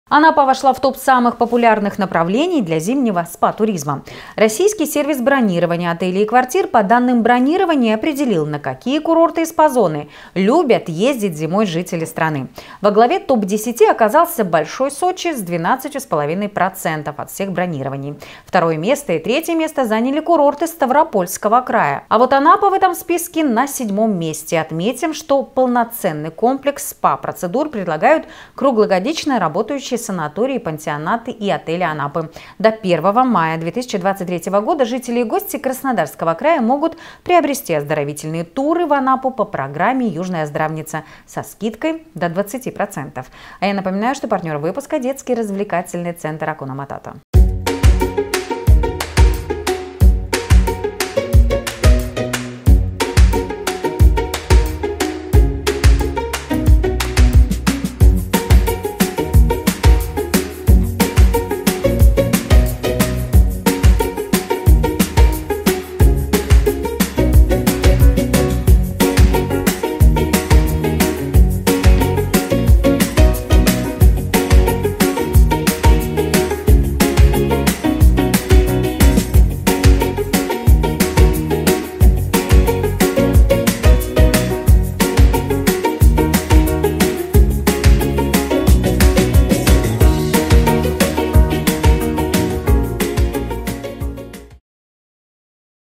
Анапа вошла в топ самых популярных направлений для зимнего СПА-туризма
Анапа вошла в топ самых популярных направлений для зимнего СПА-туризма. Российский сервис бронирования отелей и квартир, по данным бронирований, определил, на какие курорты и СПА-зоны любят ездить зимой жители страны